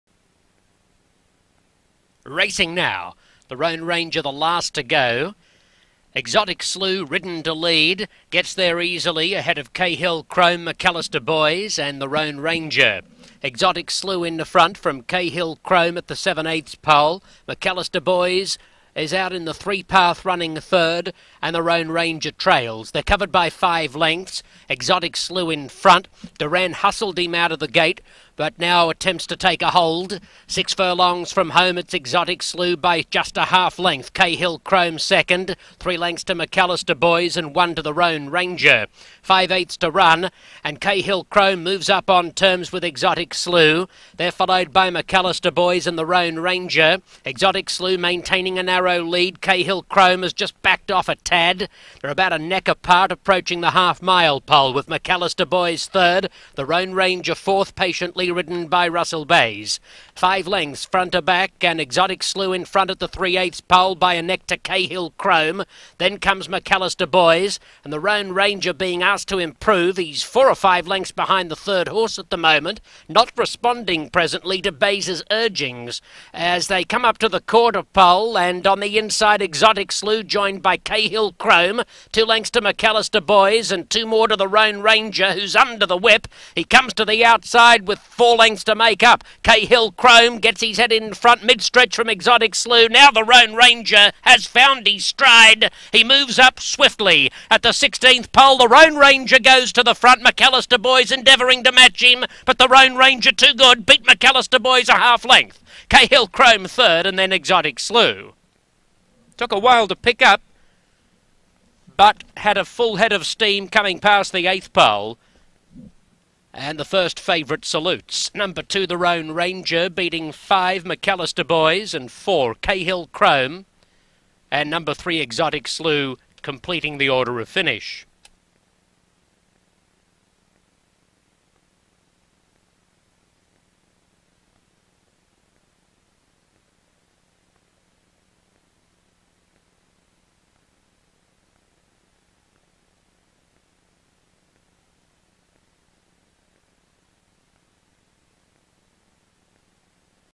Roan Ranger wins 04/26/2012
The Roan Ranger wins for Blinkers On Racing Stable on 4/26/12.